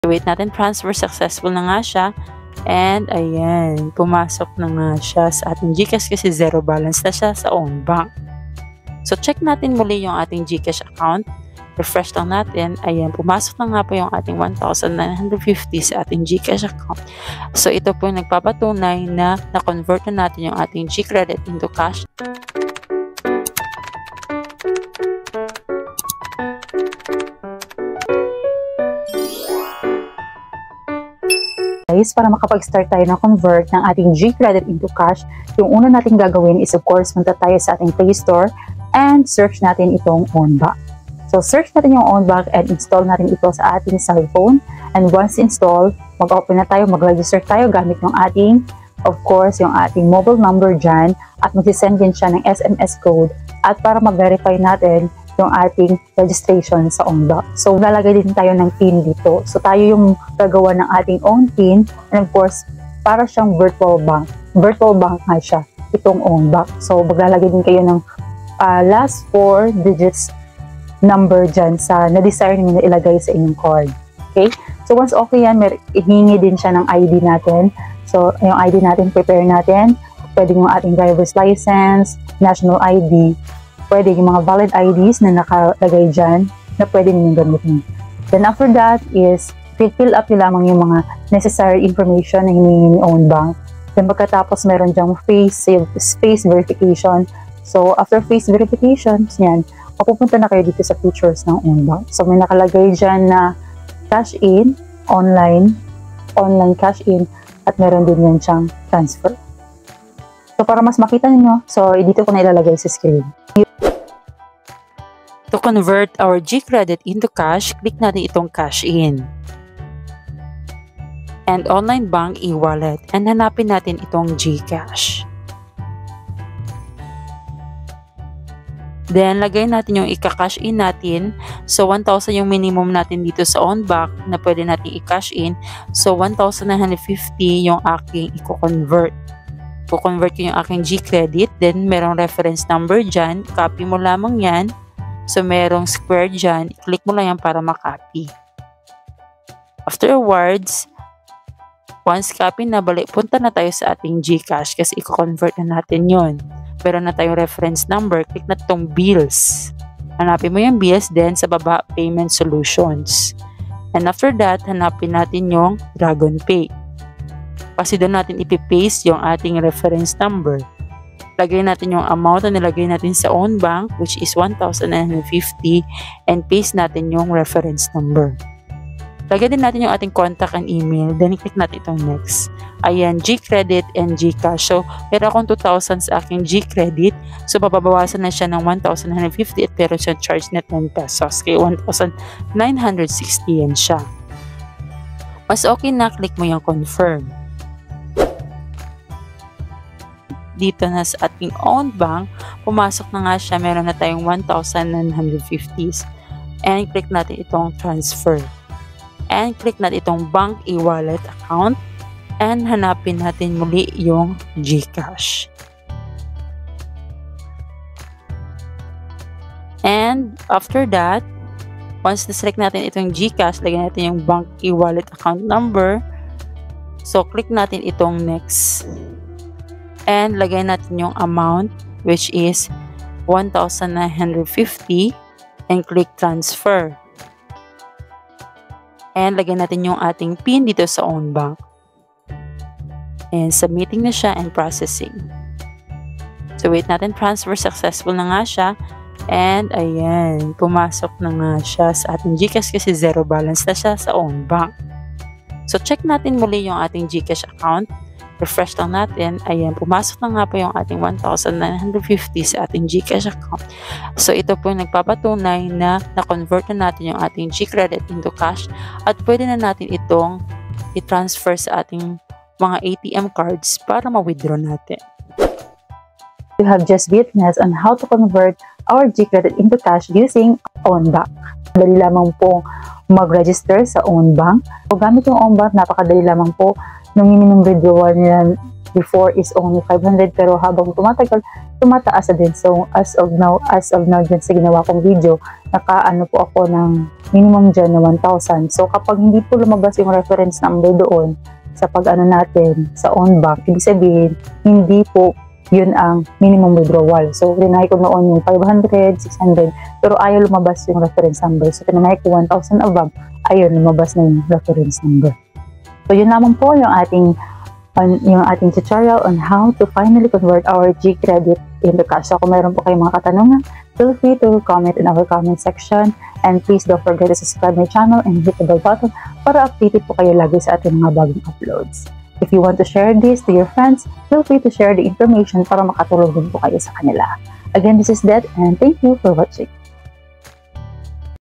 GCREDIT TO CASH 100% EFFECTIVE STEPS | Na CASHOUT KO NA GCREDIT KO
GCREDIT TO CASH possible napo at napakadali lang. 100 % Effective po itong ituturo ko na steps. WATCH AND LEARN

➡️TIKTOK HELPFUL VIDEO TUTORIALS